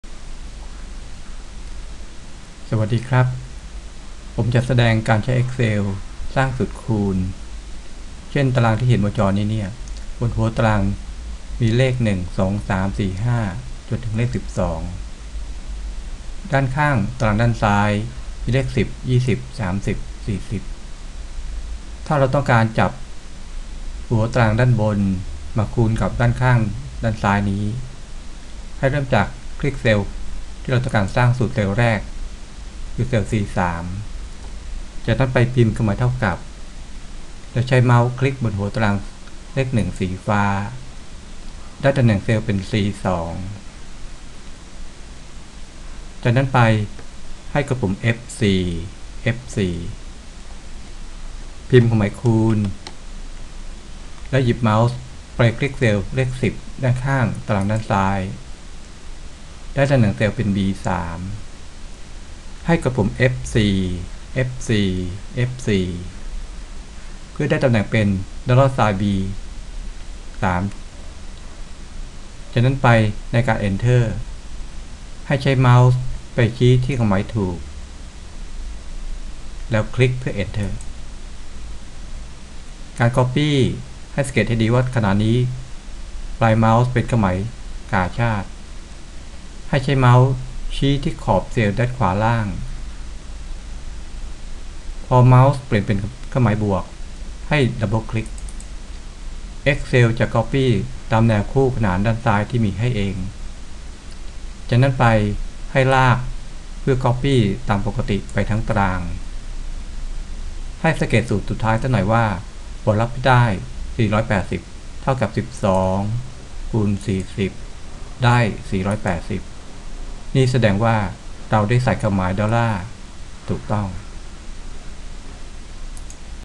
วิธีสร้างสูตรฉลาด
การสร้างสูตรฉลาด(Smart Formula)
ต้องรู้จักการกดปุ่ม F4 เพื่อกำกับตำแหน่ง Row หรือ Column ที่ต้องการควบคุมไม่ให้เปลี่ยนตำแหน่ง